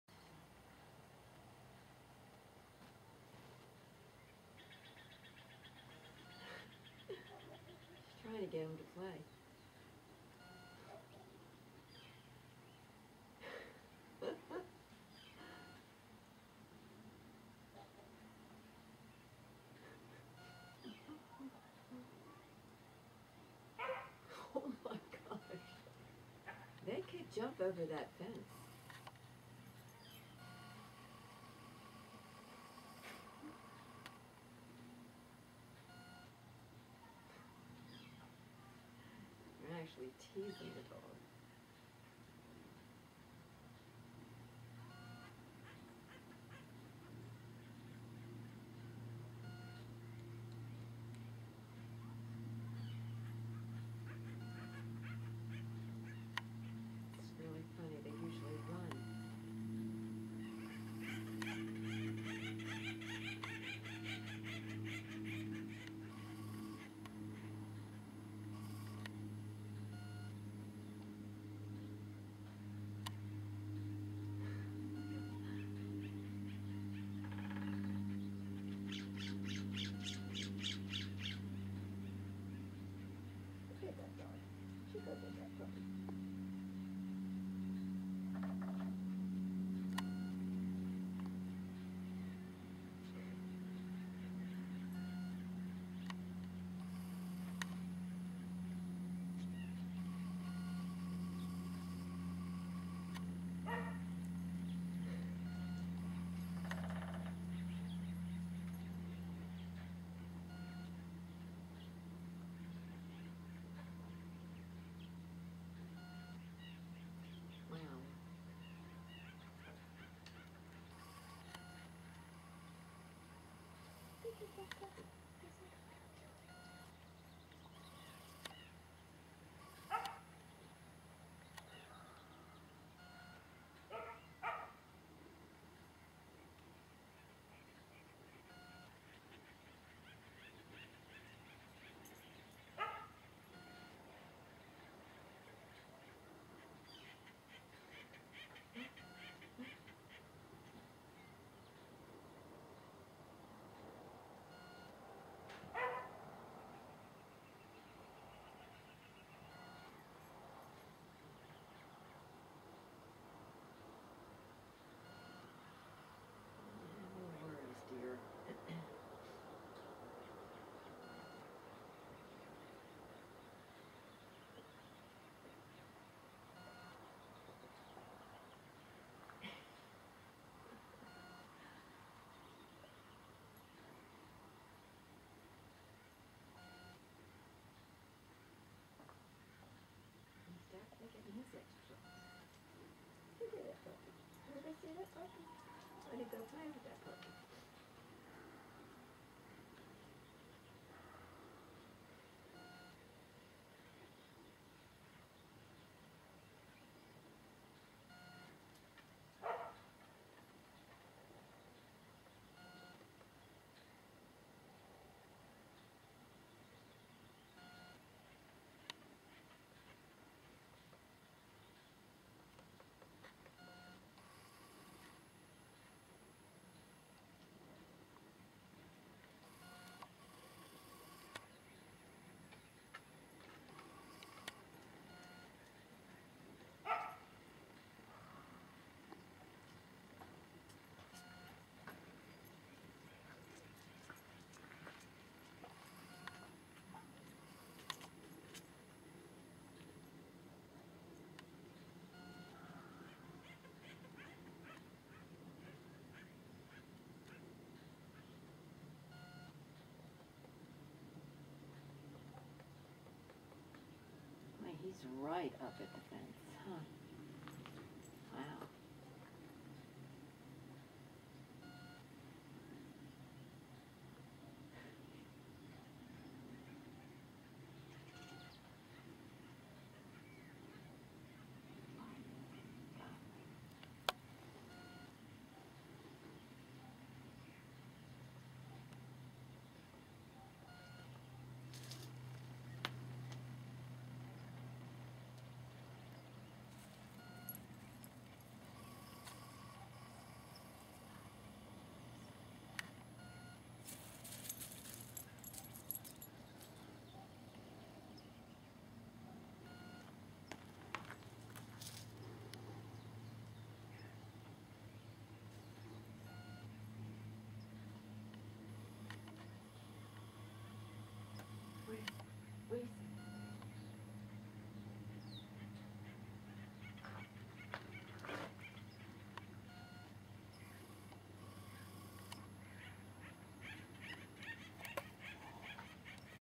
Deer meet dog
Young deer play with dog - or, is that challenge a dog?